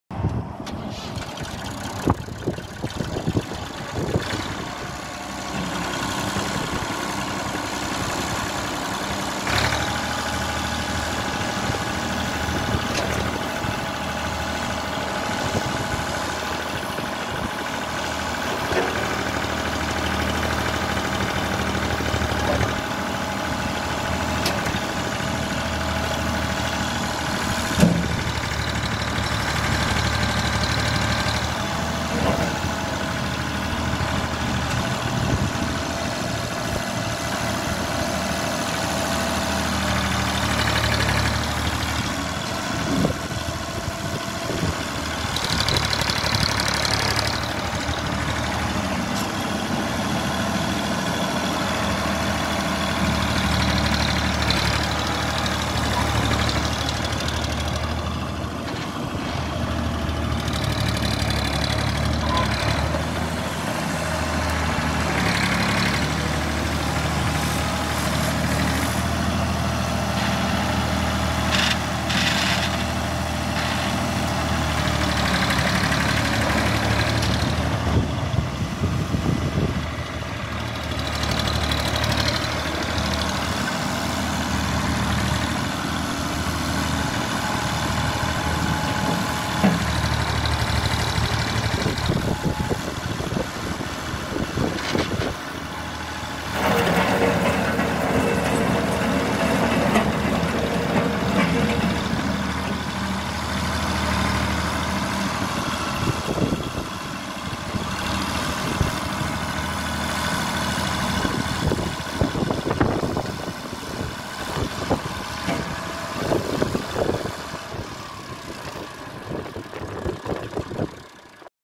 1995 MASSEY FERGUSON 240. $19,90
OROPS
4WD
BUSH HOG 2445QT LOADER
8 speed transmission
2 REAR REMOTES
2,655 hours